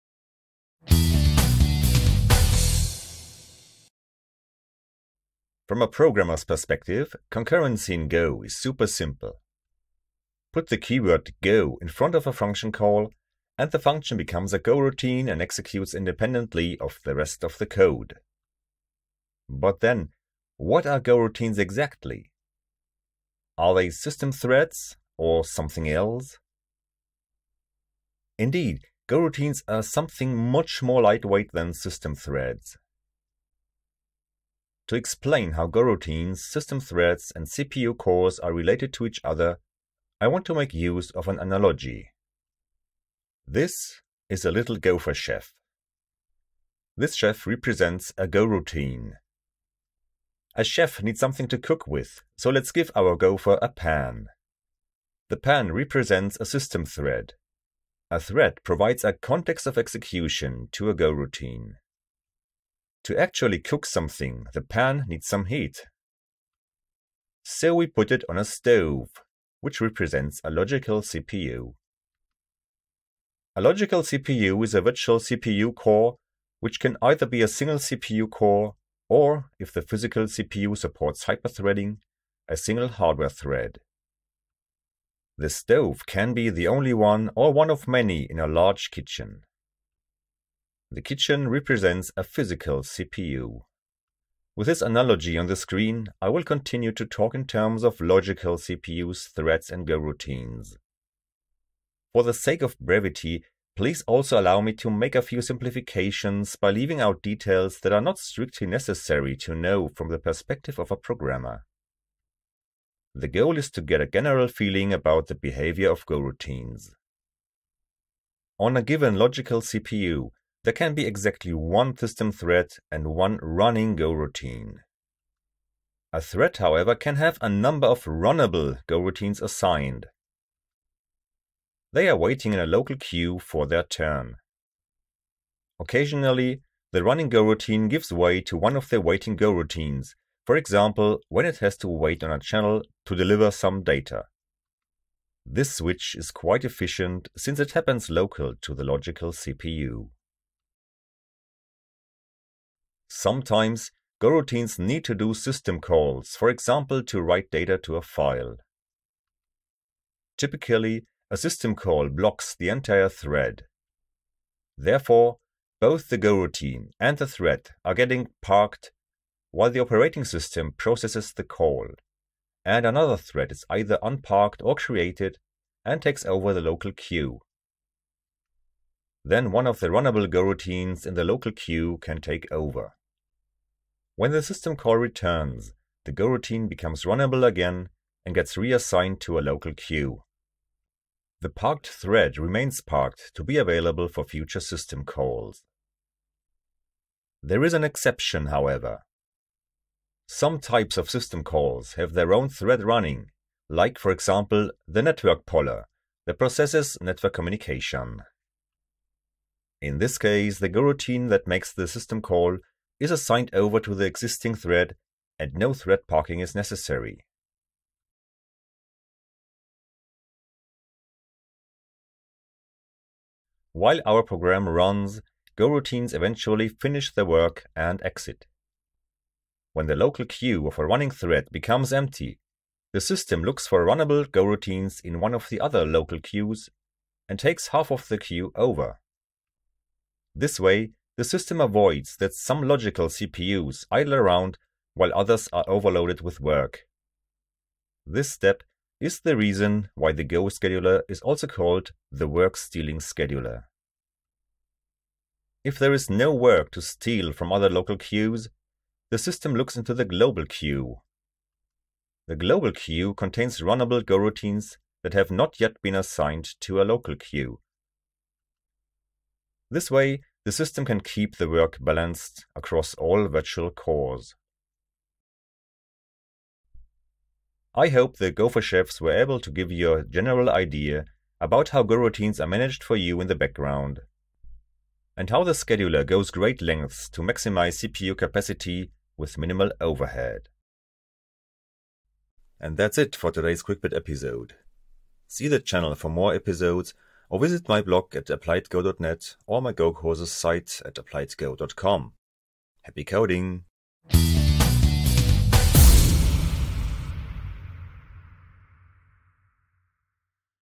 Applied Go Quick Bits 007: Are goroutines threads?
Struggling with concurrent programming? Take a look at the Concurrency Deep Dive course

Goroutines are Go's central concurrency mechanism. They are so super easy to create an manage that you might wonder how they are implemented. Are they plain old system threads in disguise? Not at all. This video shows how goroutines are mapped onto threads, and how the system does the heavy lifting behind the scenes to manage hoards of goroutines, get blocking system calls out of the way, and keep up the flow through the work-stealing scheduler.

Further readings about the Go scheduler:

And Ardan Labs even have a 3-part series on the topic

This is the Applied Go Quick Bits series, delivering bite-sized Go snacks right to your screen.

The Gophers in the video are created by me, based on the original Gopher by Renée French.

For the other images I used, big shout-outs to these artists on Pixabay:

OpenClipart-Vectors – fireplace, pan, phone box, table
katya-guseva0 – flames
Coverr-Free-Footage – pan contents
Diskette96 – chef's hat
simisi1 – cooking spoon
Myriams-Fotos – threads and needle
Kirgiz03 – grumpy cat
Hans – feather